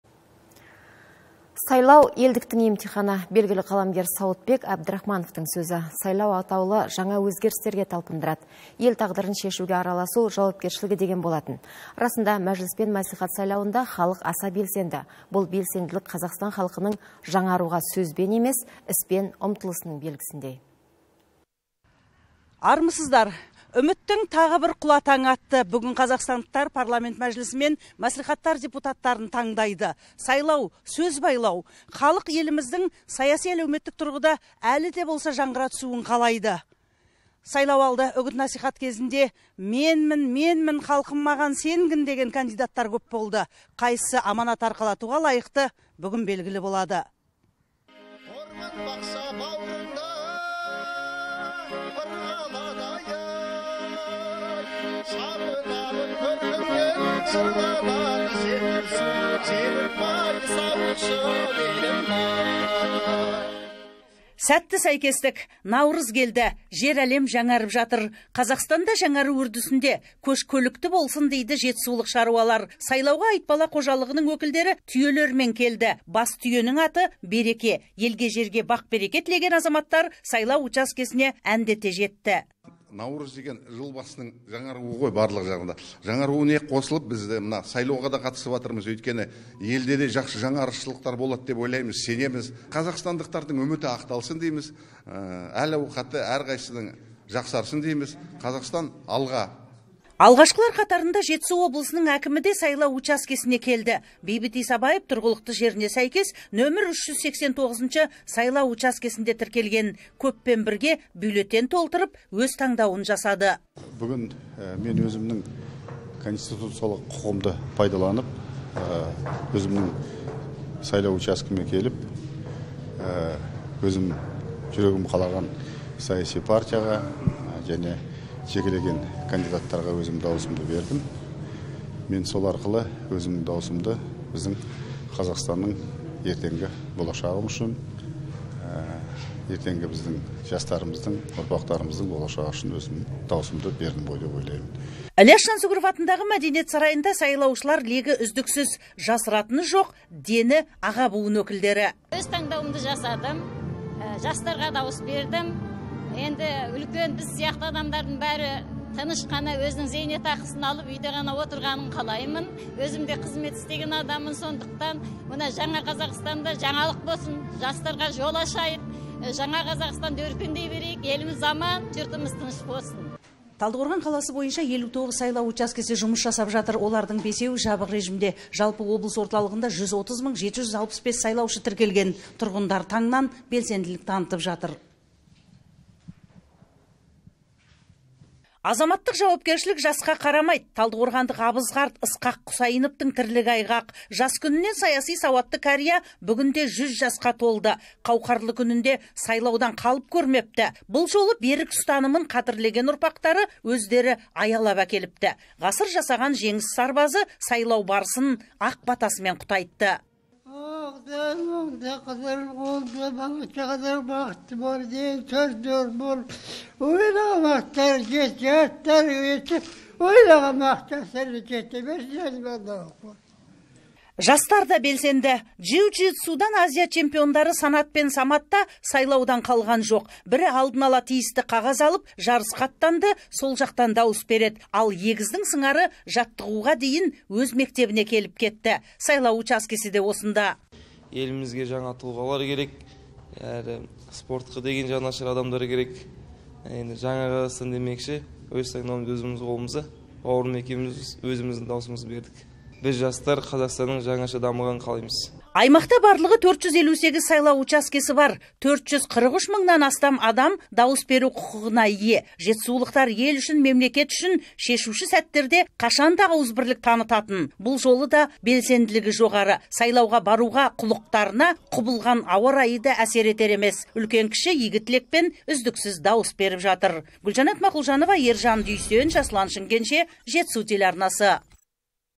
Жетісу облысының әкімі өз таңдауын жасады
"Сайлау- елдіктің емтиханы". Белгілі қаламгер Сауытбек Абдрахмановтың сөзі. Сайлау атаулы жаңа өзгерістерге талпындырады. Ел тағдырын шешуге араласу жауапкершілігі» деген болатын. Расында, Мәжіліс пен мәслихат  сайлауында халық аса белсенді. Бұл белсенділік - Қазақстан халқының жаңаруға сөзбен емес, іспен ұмтылысының белгісіндей.